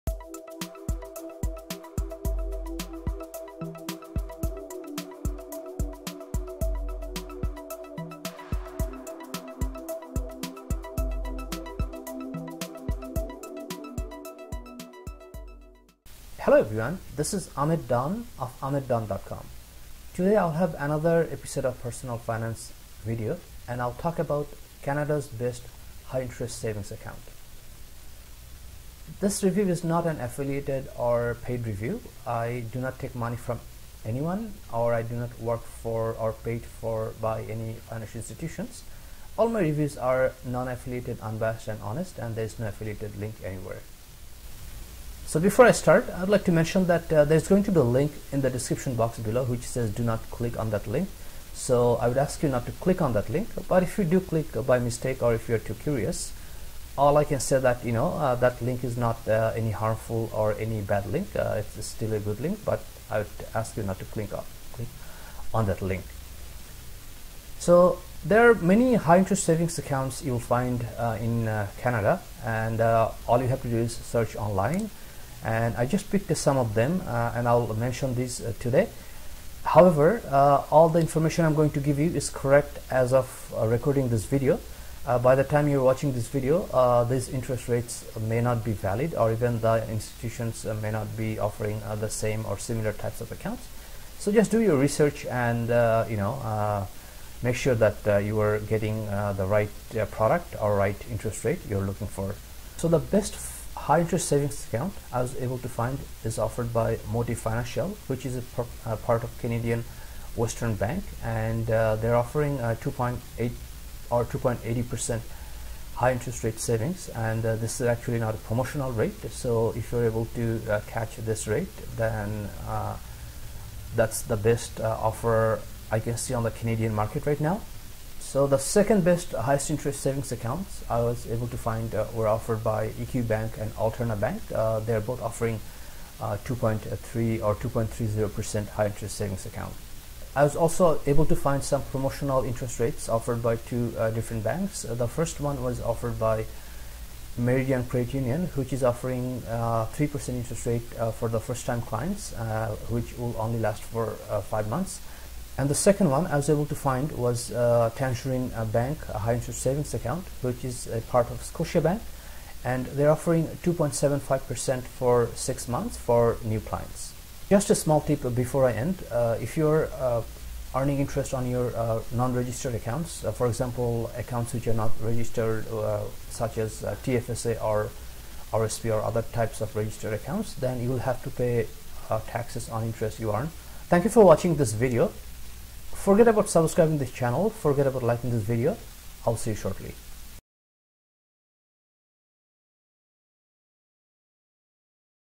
Canada's Best High-Interest Savings Accounts | Financial Author Ahmed Dawn
Canada’s Best High-Interest Savings Accounts

In searching for Canada’s top high-interest savings accounts, I came across these accounts mentioned below. Some of these rates are promotional and won’t last long.

The best interest rate currently available in Canada:

Motive Savvy Savings Account = Interest Rate 2.80%

2nd best interest rates in Canada:

EQ Bank Savings Plus Account
and
Alterna Bank High Interest eSavings Account = Interest Rate 2.30%

Best Promotional Interest Rate in Canada:

Tangerine = 2.75% first 6 months
Meridian Credit Units = 3.00% interest for 5 months

Always do your research and choose the products and interest rates that suit you most.

Financial author Ahmed Dawn's insights.

Leaked - Huge Changes Are Coming to the Scotiabank Gold  American Express Card